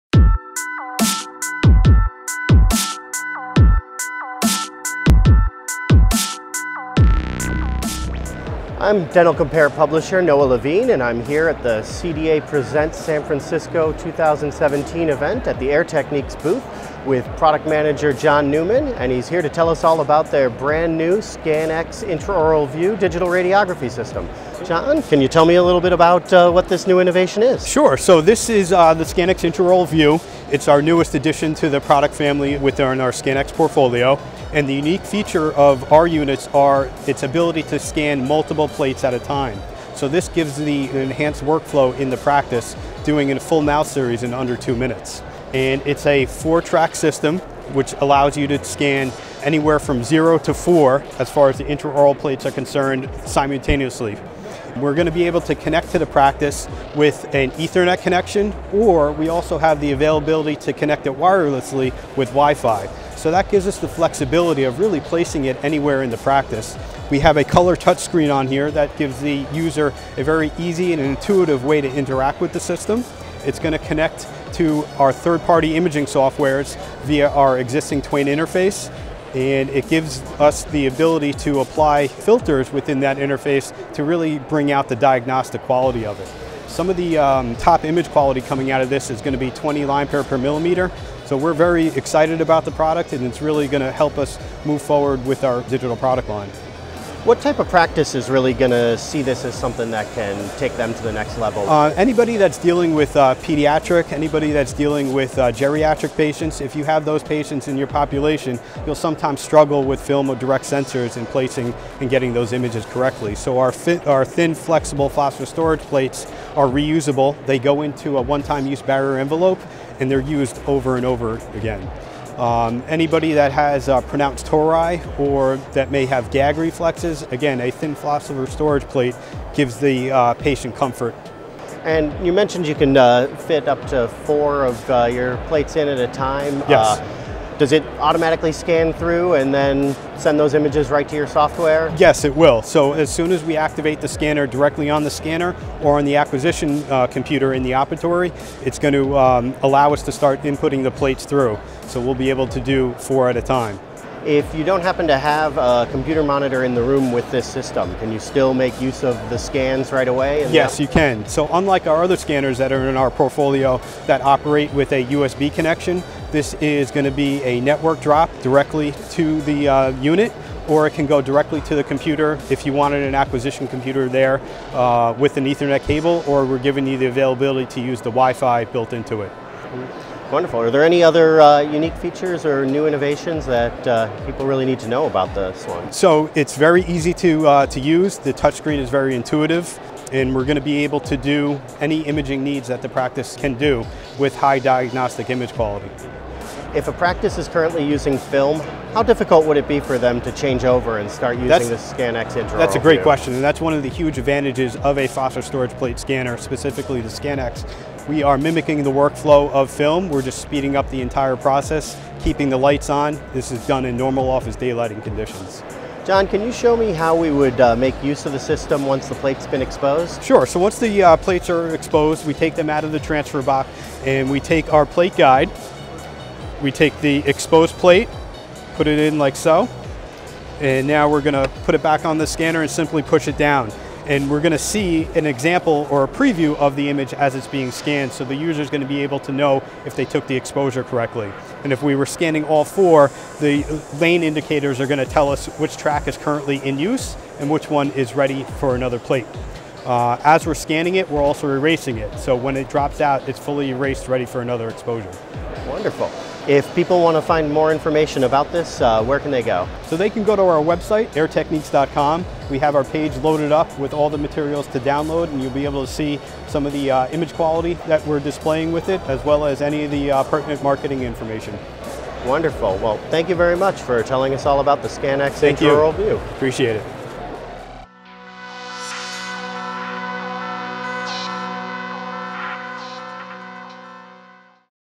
ScanX Intraoral View from Air Techniques
ScanX Intraoral View Digital Radiography System from Air Techniques. WIFI and Ethernet connections. Scan up 4 plates simultaneously. Excepts all intraoral sizes 0 through 4.